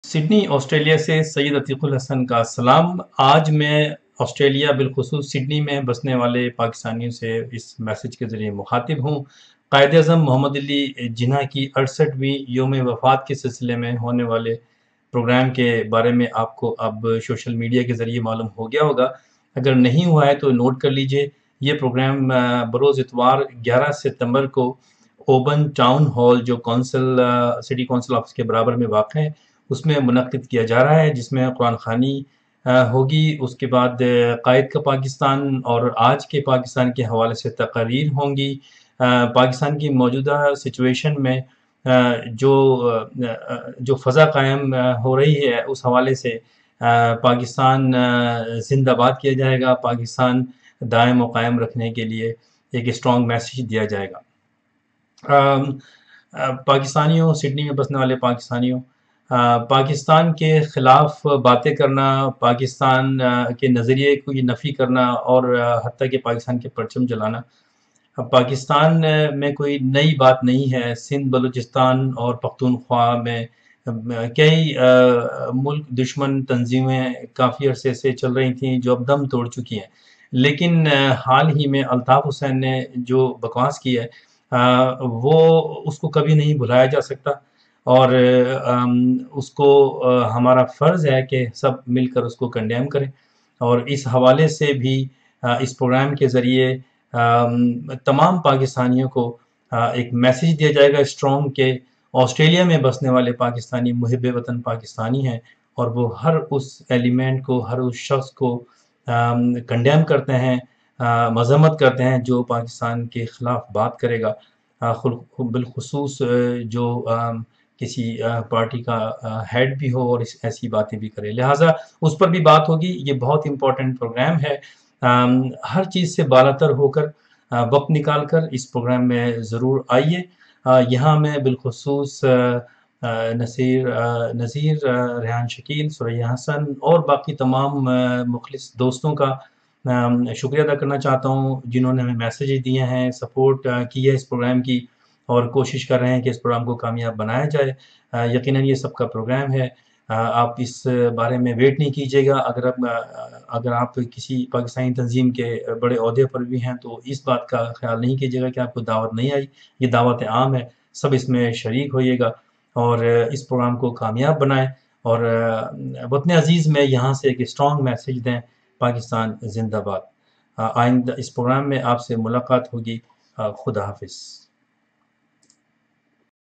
Quaid e Azam 68th Death Anniversary Day Event in Sydney
Message from Syed Atiq ul Hassan in context of condemnation to Altaf Hussain's statements against Pakistan.